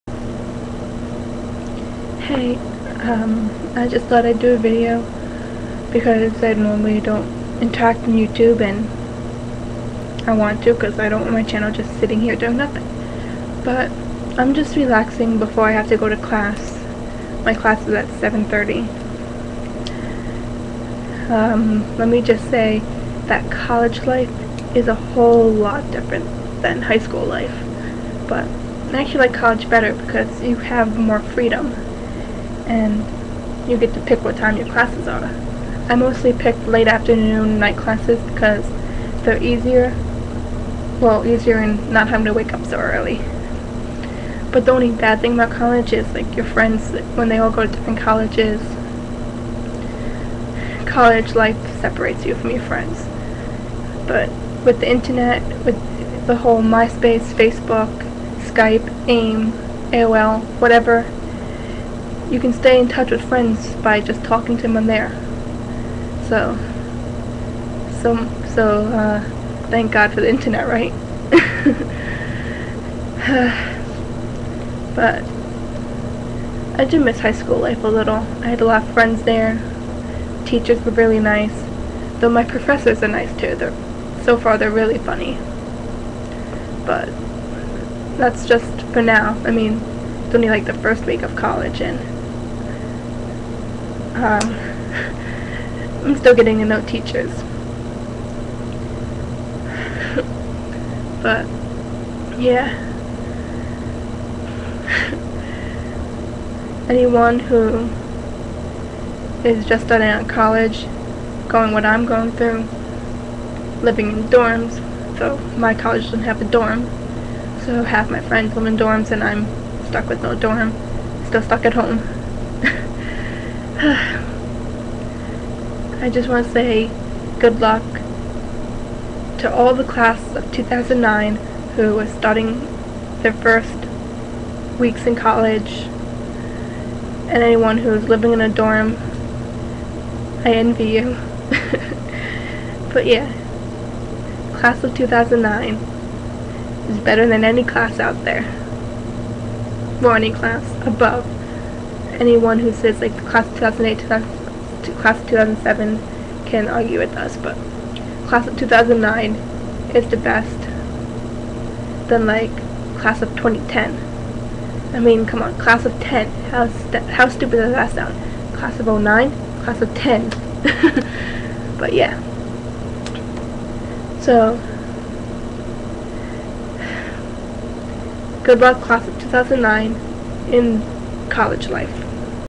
Talking about College life
new life in college is always tough and now I'm expriencing it along with all my class of 2009 friends hope all new college students are enjoying their new life like I am